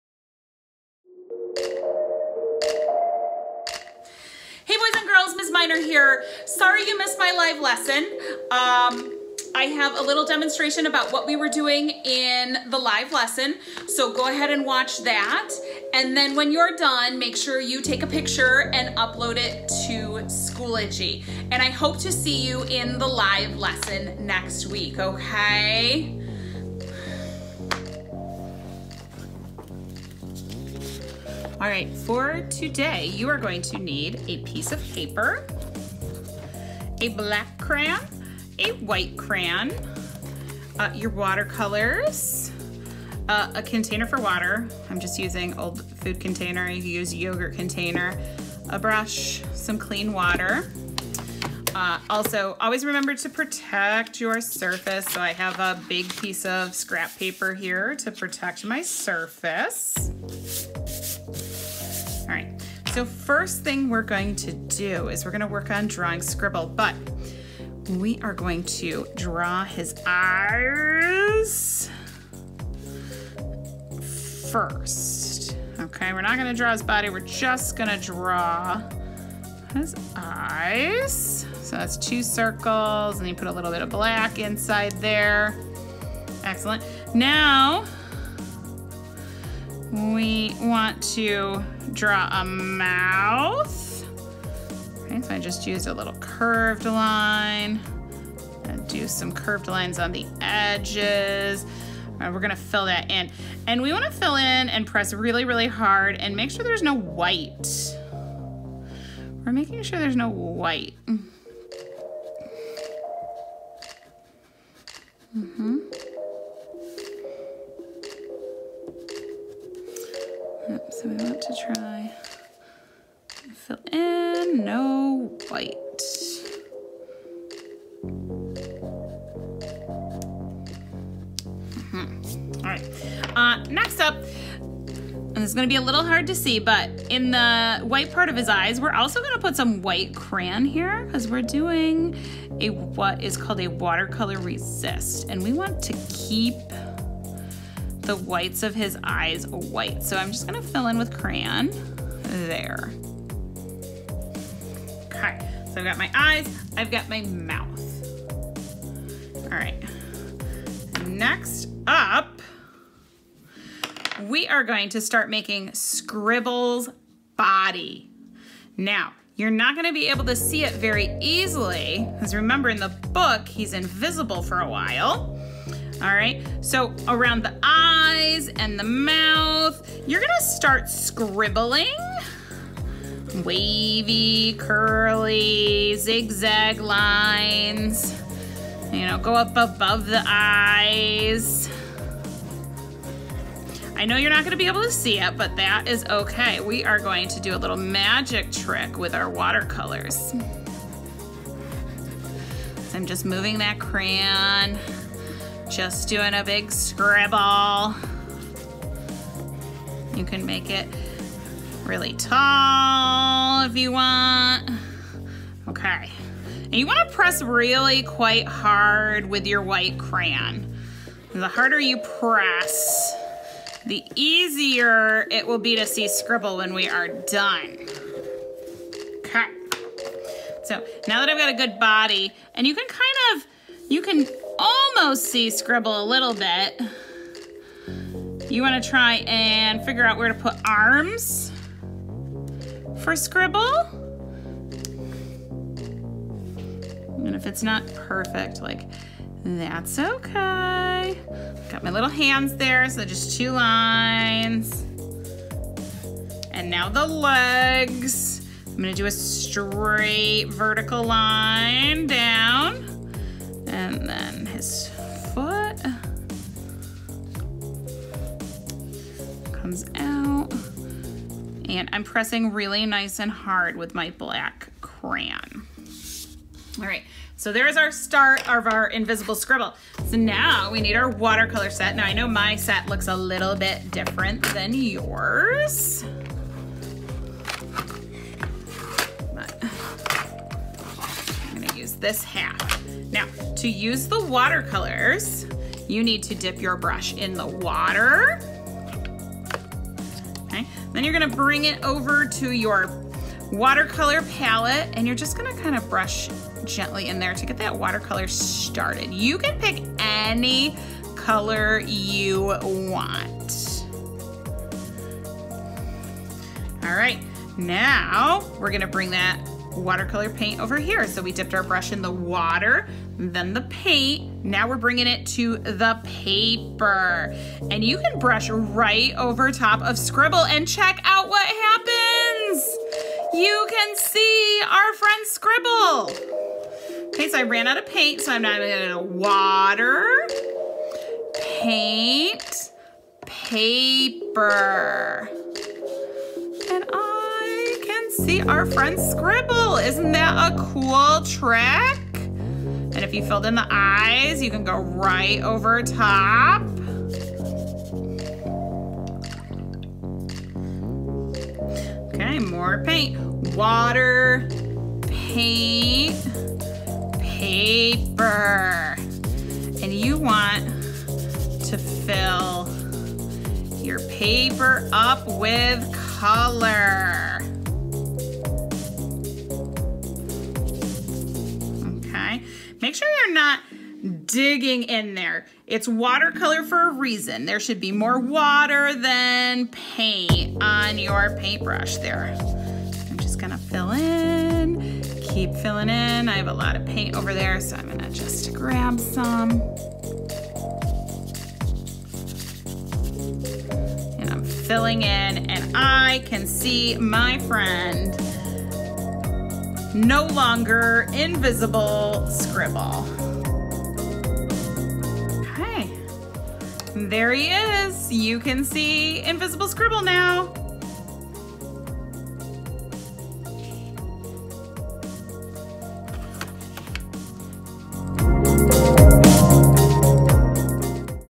Invisible Scribble Demo
Please watch the read-aloud of Invisible Scribble by Diane Alber first

After reading Invisible Scribble by Diane Alber I show my students how to use watercolor resist to make their invisible scribble visible.